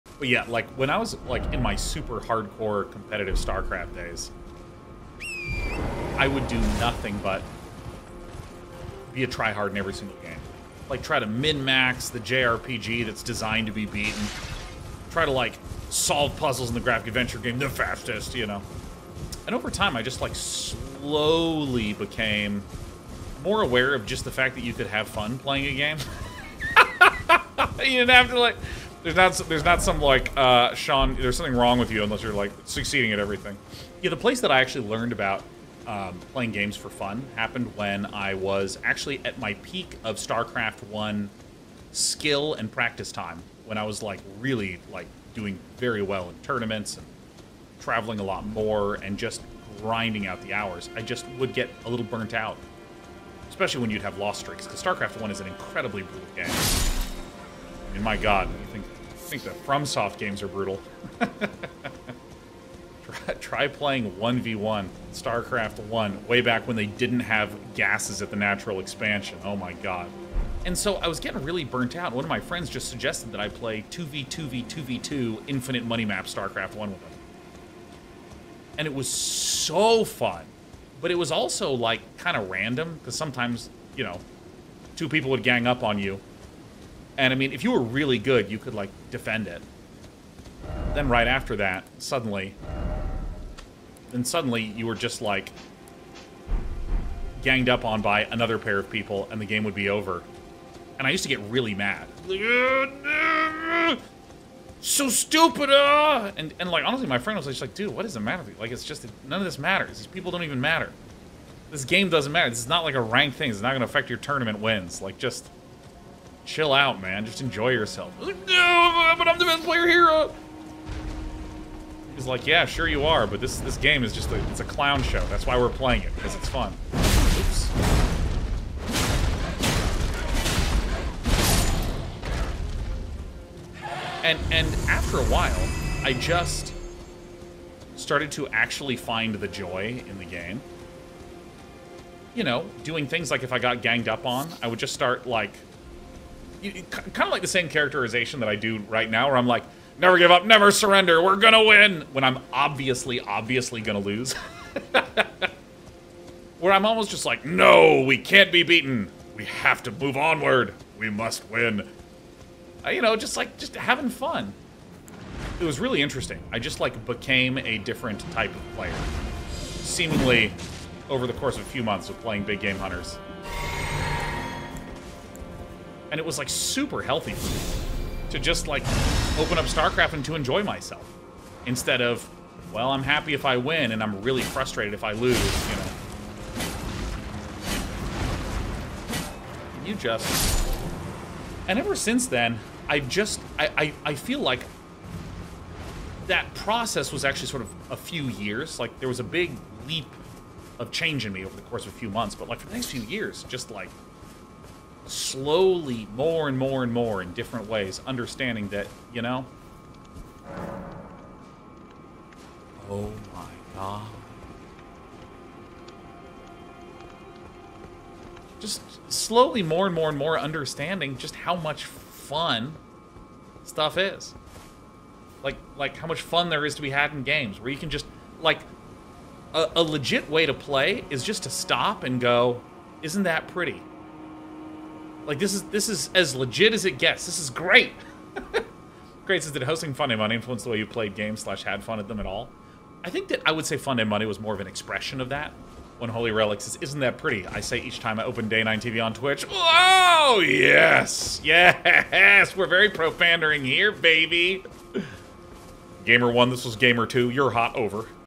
Finding the Joy in Games | Day9 Mini Rant
This is just a quick little talk about how I started finding the joy in video games.